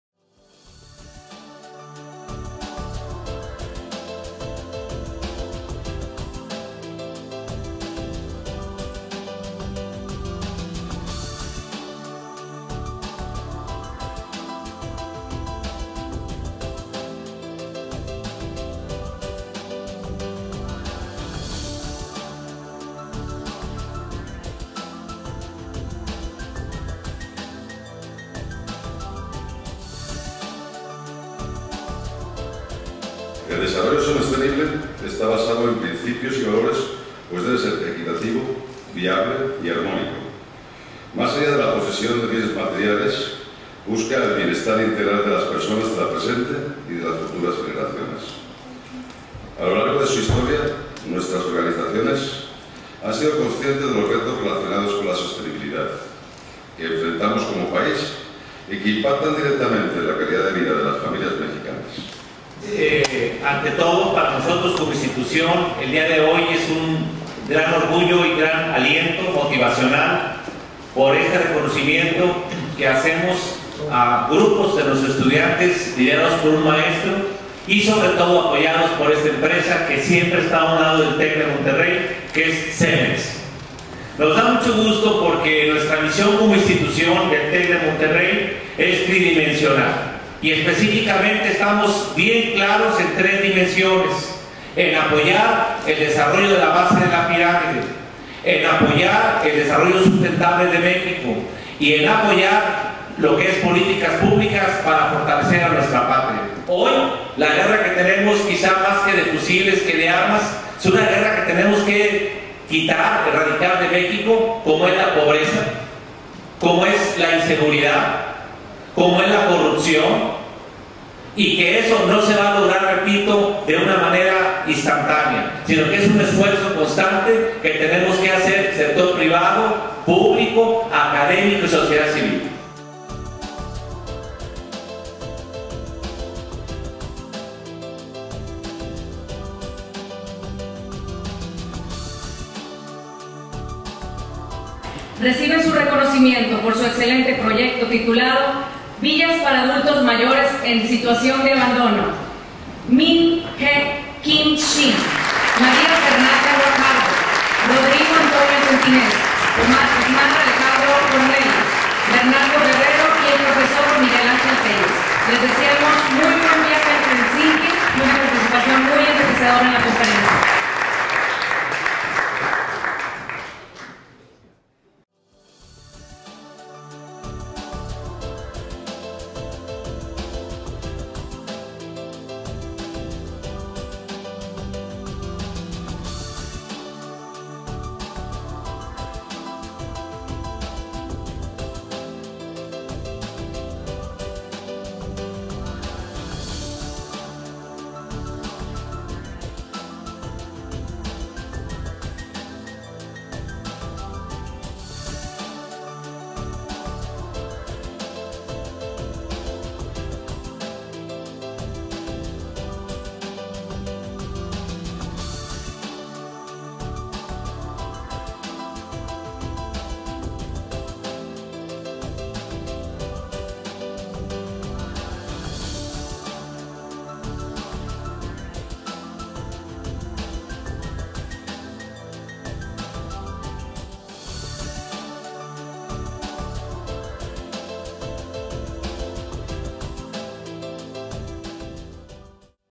Ceremonia de Premiación Premio CEMEX-TEC Transformando Comunidades
El Tecnológico de Monterrey y CEMEX entregaron el Premio CEMEX-TEC Transformando Comunidades, en el que participaron casi 200 alumnos en 51 equipos de los campus del Tecnológico de Monterrey.